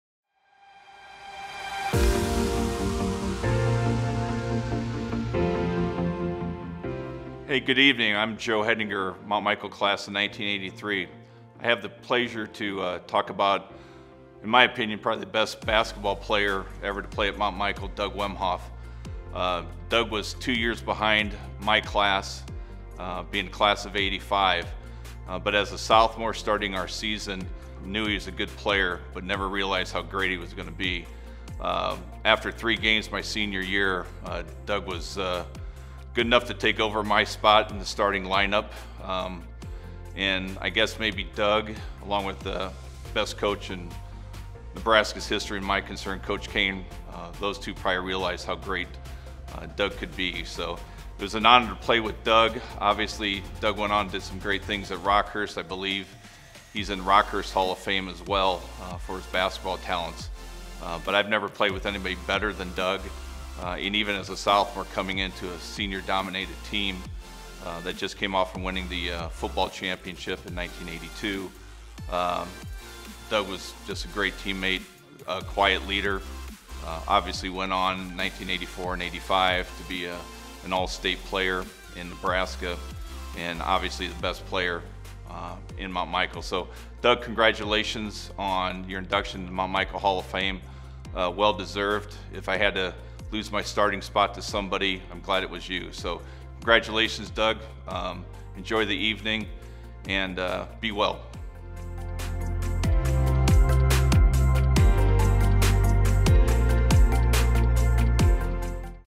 Doug Wemhoff 2022 Hall of Fame Recipient
Doug Wemhoff- Doug Wemhoff is one of the most accomplished athletes in Mount Michael history. As a three sport athlete Wemhoff helped guide the Knights to the state championship in basketball 2 times in his last three years. His senior year, he led Mount Michael to a 22-2 record and a 22 point average margin of victory for the season. In the state tournament he scored 83 points with 26 of them coming in the Knights 53-46 win over Valentine. He was a multiple time All State Class B selection and averaged 23 points and 11 rebounds per game his senior year. In 1985 he was named to the first team of Nebraska’s All Super State Team and is the only Mount Michael basketball player to have ever earned this honor. Wemhoff still holds the school records for points in a season (559), rebounds for a career (661), rebounds for a season (278), rebounds for a game (19), career field goals (493) and field goals for a season (293). His senior track season, Wemhoff won the Class B discus title with a throw of 165 feet. He also set a school record of 172 feet, his record still stands. After leaving Mount Michael Wemhoff competed in basketball for Rockhurst University where he finished his career with 2,063 points and 887 rebounds. In 1989, during his senior year, he was named the Most Valuable Player for Rockhurst and was an NAIA All-District selection. Upon graduating from Rockhurst, he earned his juris doctorate from University